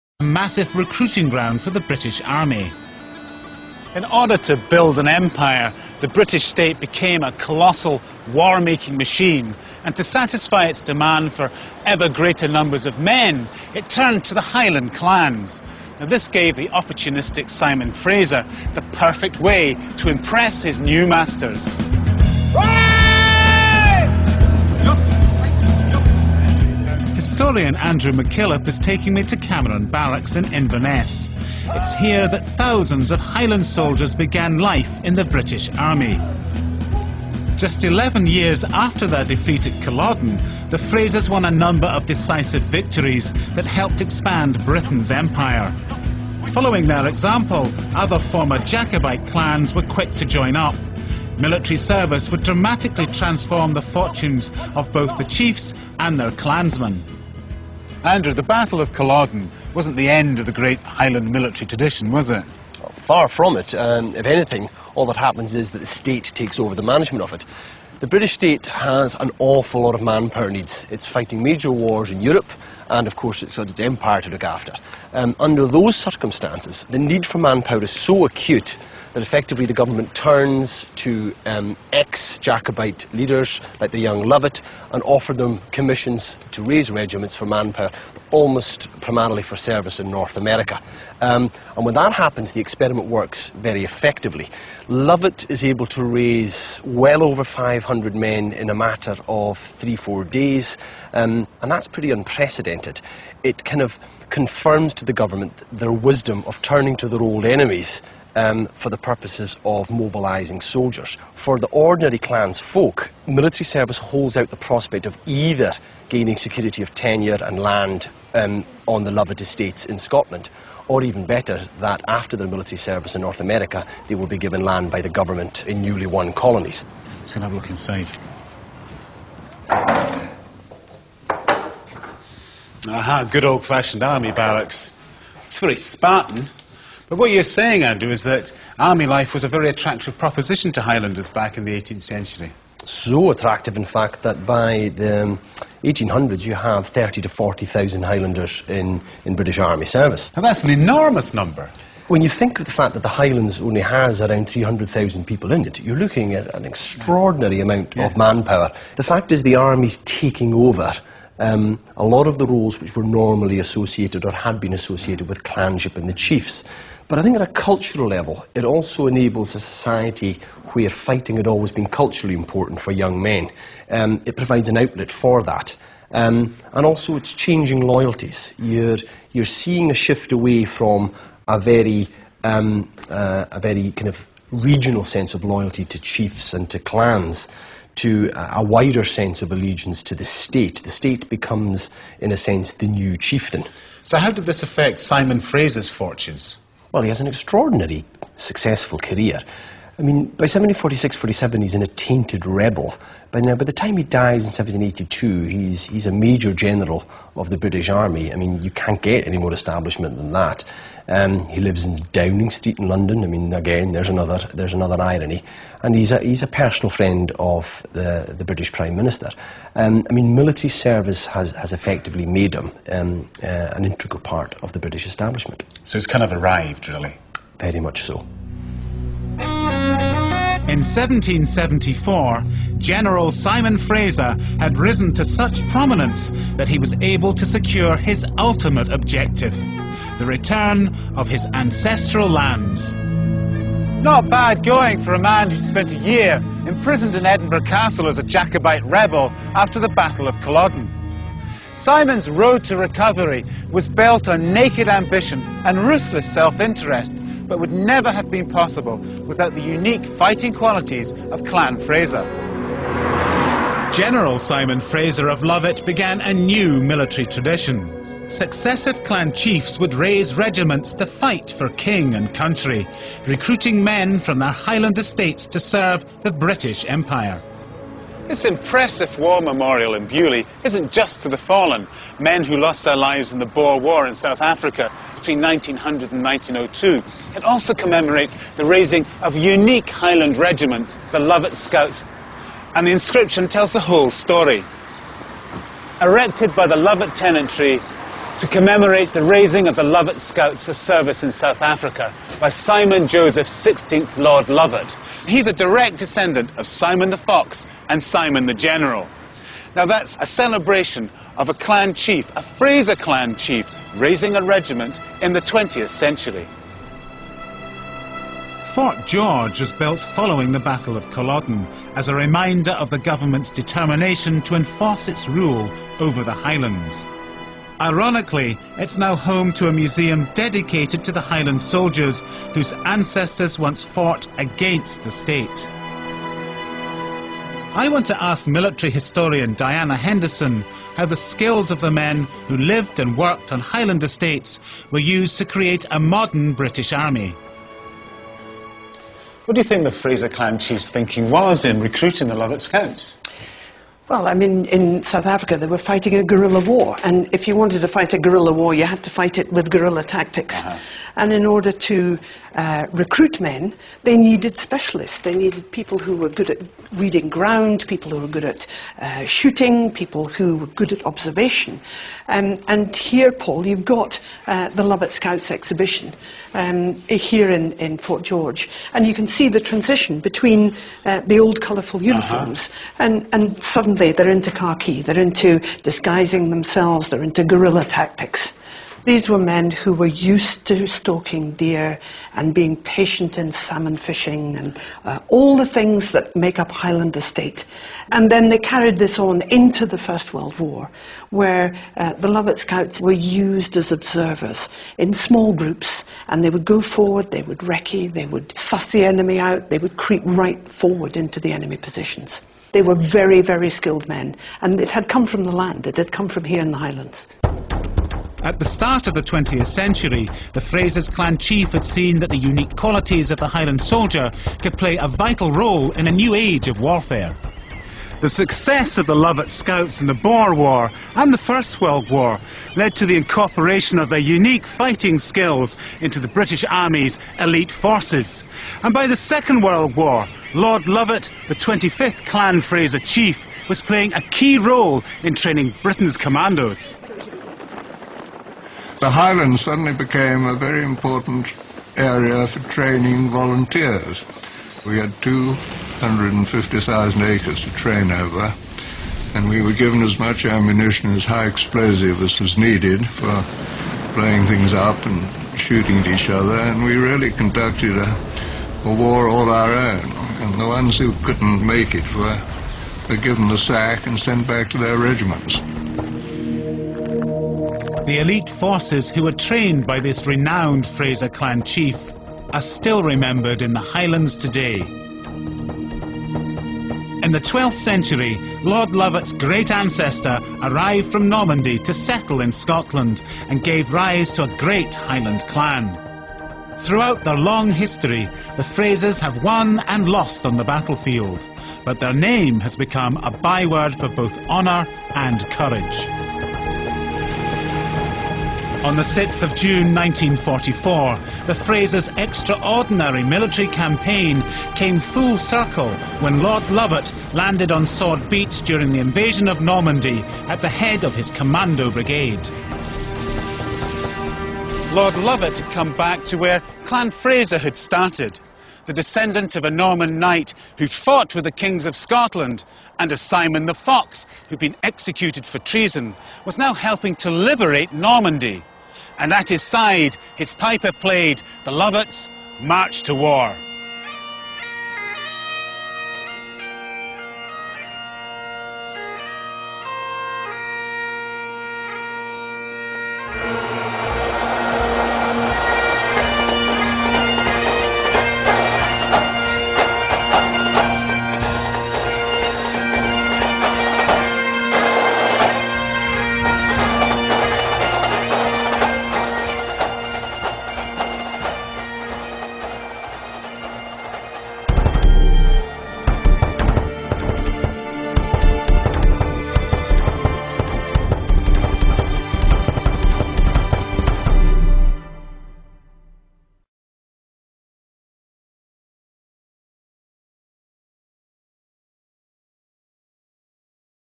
Clan Fraser of Lovat history, part 3 of 3
A documentary about the Fraser of Lovat highland clan.